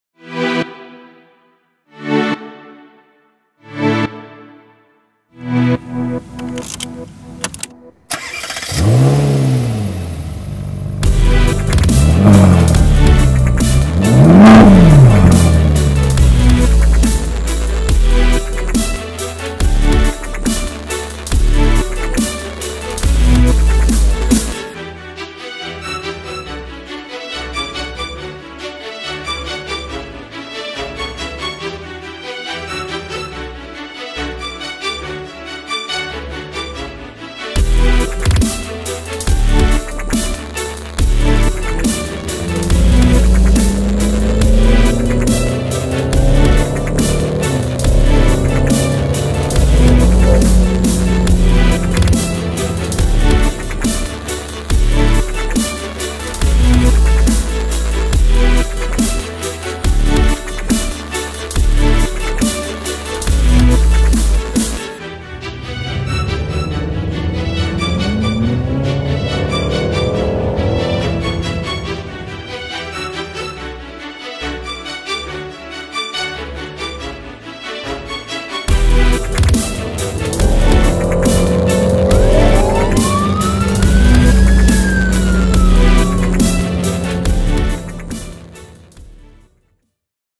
Ferrari California | Forgiato Wheels
White Ferrari Califorina with custom painted Aggio-ECL Forgiato Wheels to match (21" front, 22" rear) with floating center caps, Pirelli tires, 3M roof wrap, and SunTek window film